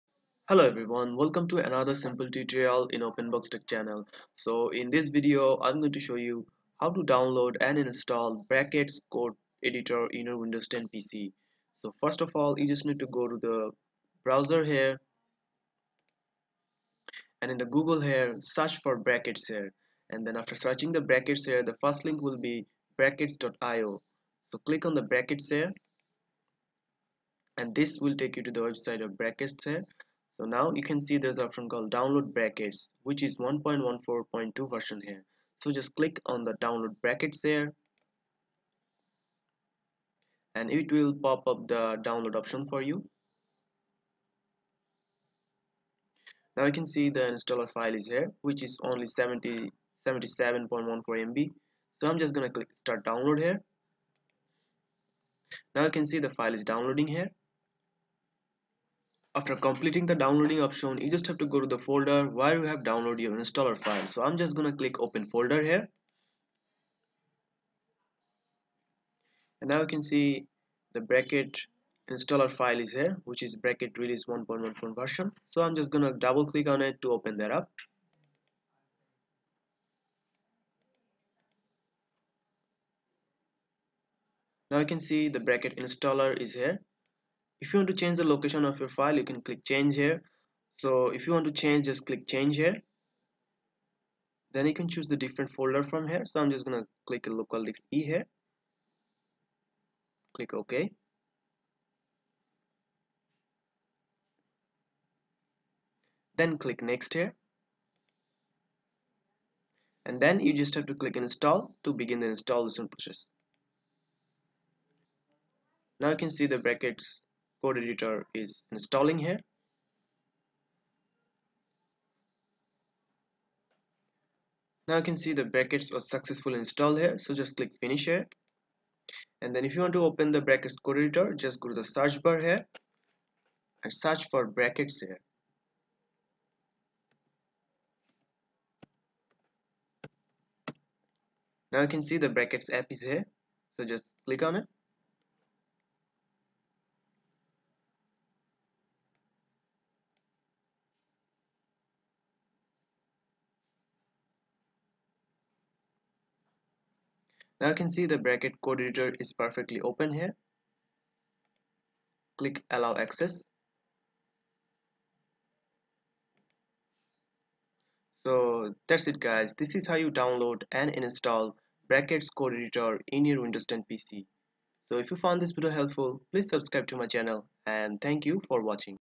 How to install brackets code editor in windows 10
in this video How to download and install brackets code editor in windows 10

Brackets is a open source code editor with a primary focus on web development.